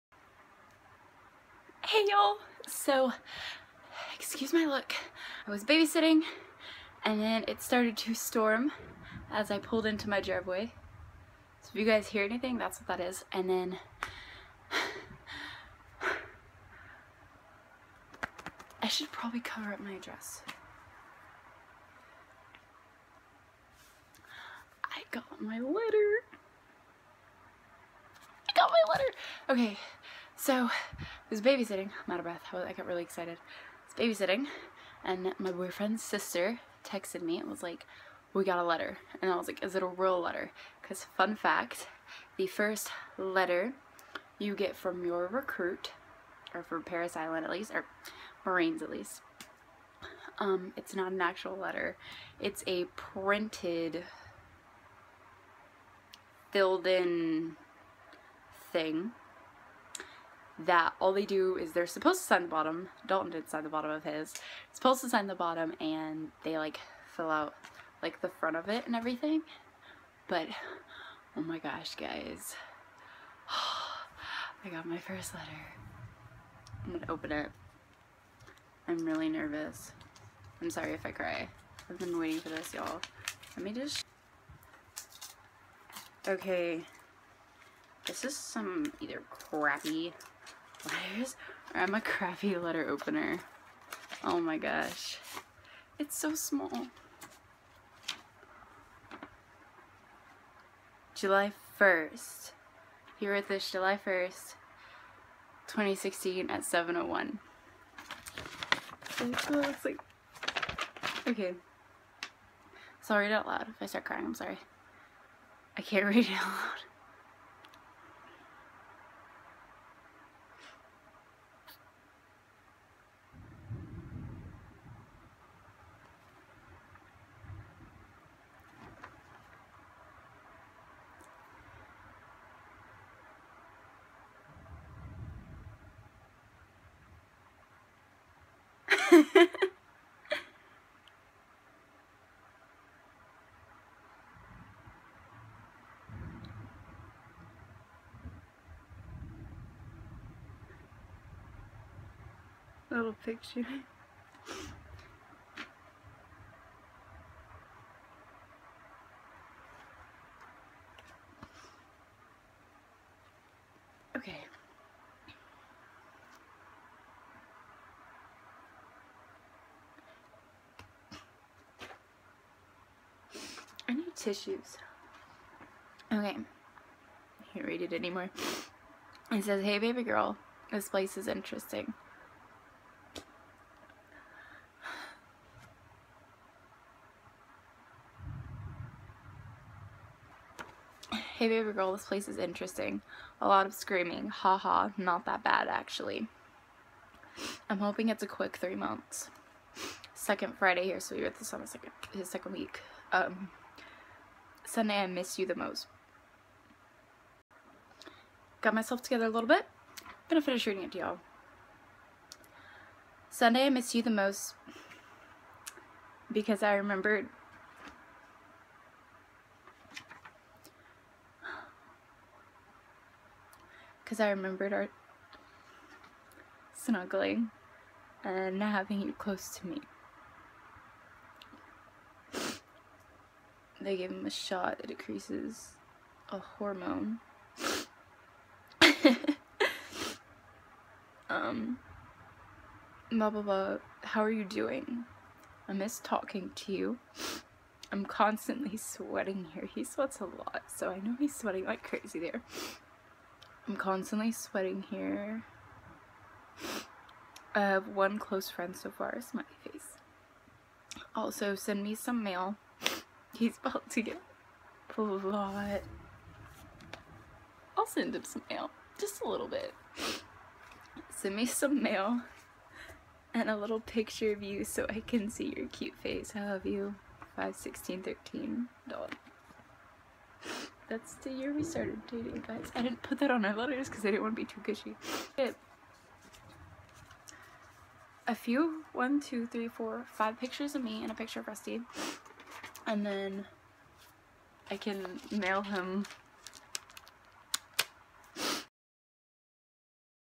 Opening My First Letter!!
This is really crappy and I'm out of breath so I'm sorry about that but I can't recreate a video like this. So this is me opening my first letter from boot camp from my baby. Hope yall enjoy.